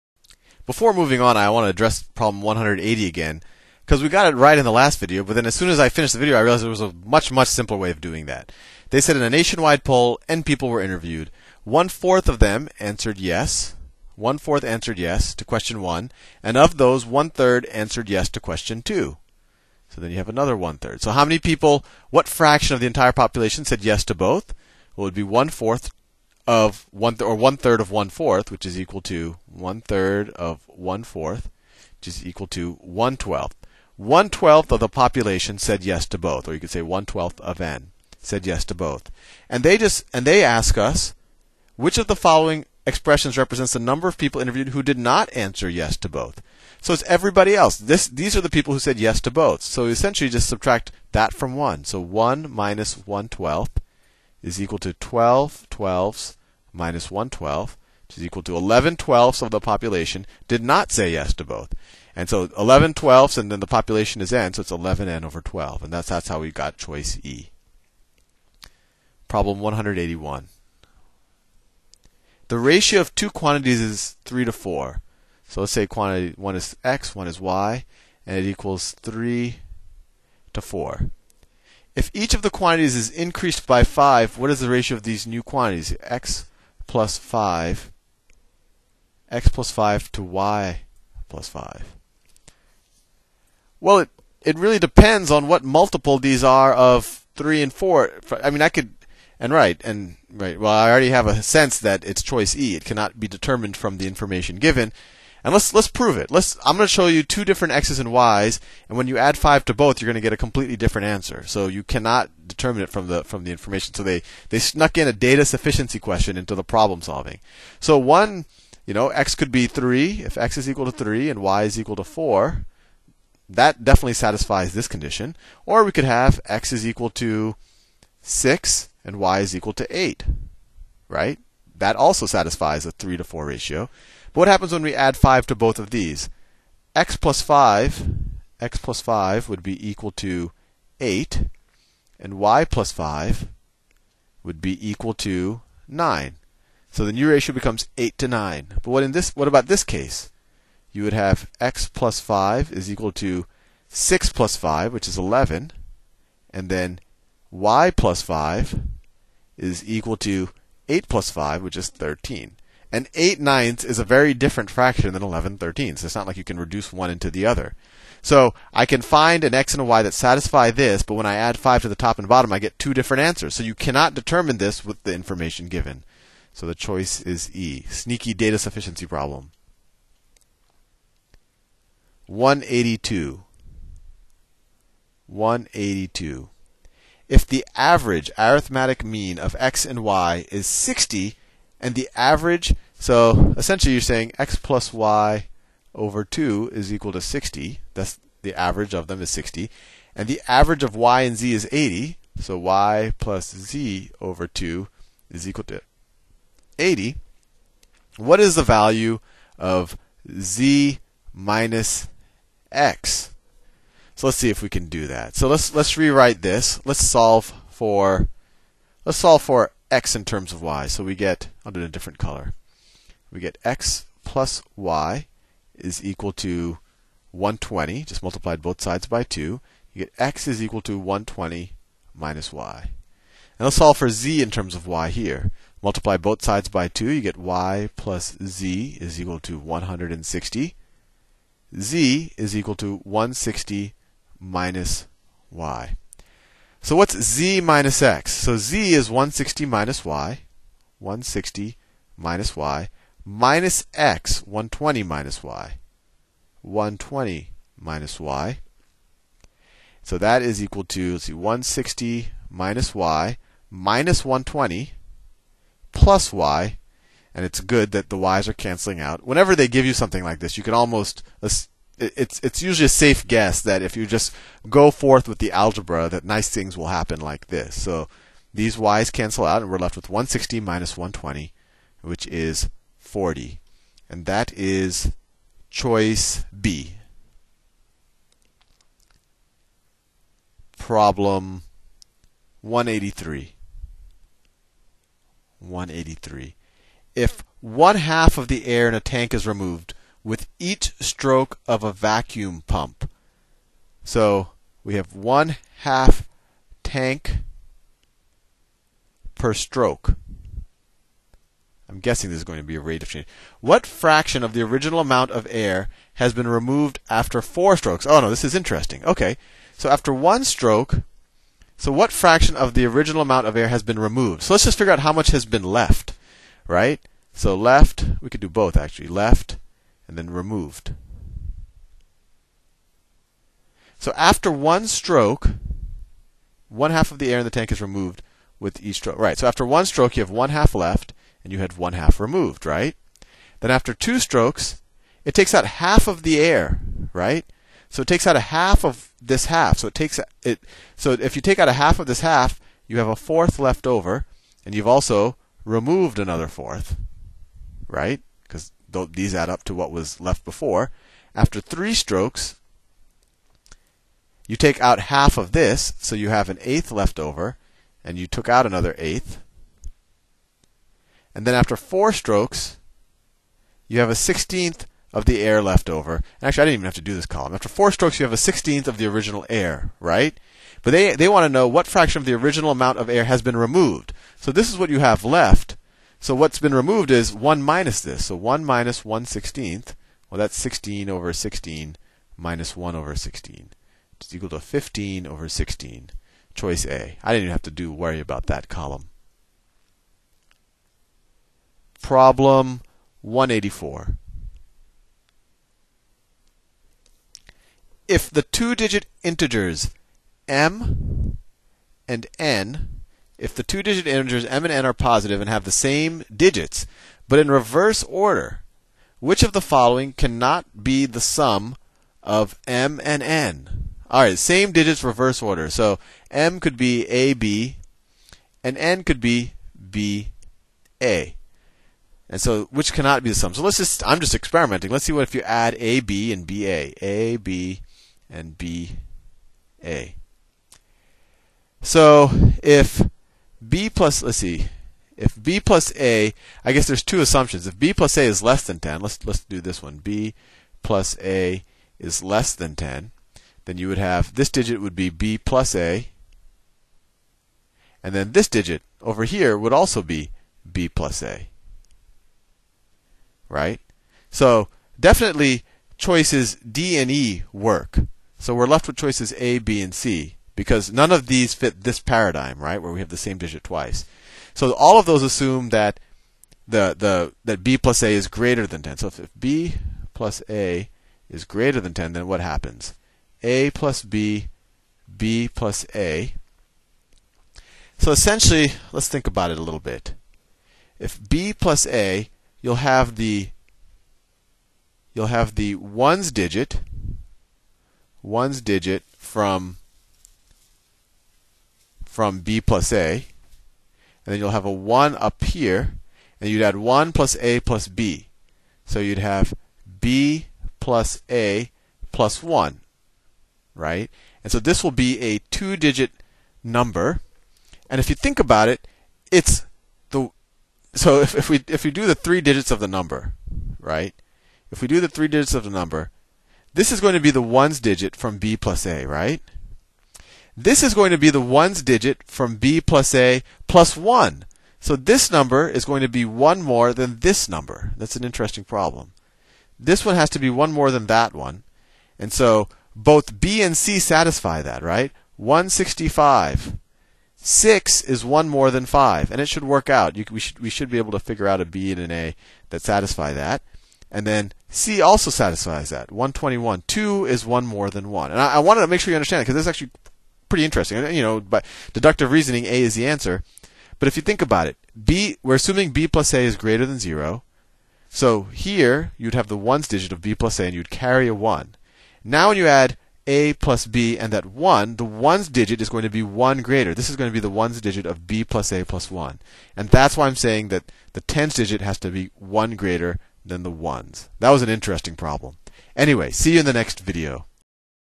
GMAT: Math 36 | Problem solving | Khan Academy
Courses on Khan Academy are always 100% free. Start practicing—and saving your progress—now

180(simpler)-184, pgs. 176-177. Created by Sal Khan.

GMAT on Khan Academy: Sal works through the 249 problem solving questions in chapter 5 of the the 11th edition of the official GMAC GMAT Review (ISBN Number: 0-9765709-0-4 published in 2005)